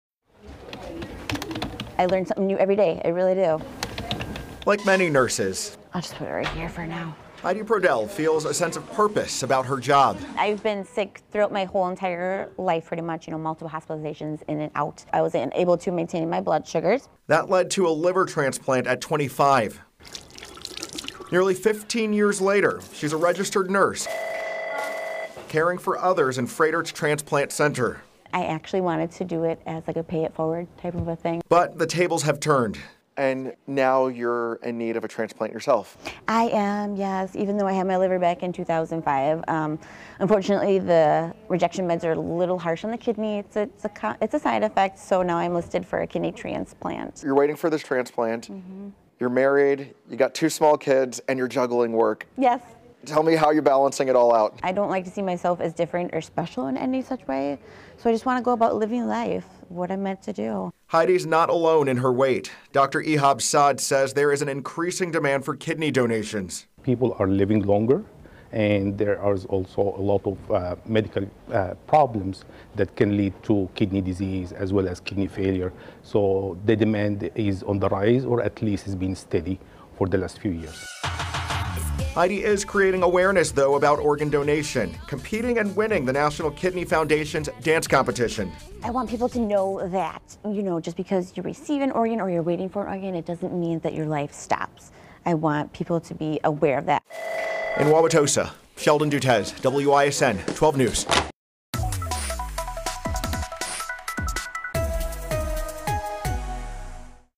Transplant nurse in need of transplant herself
Transplant nurse, Heidi Prodehl is raising awareness for patients like her.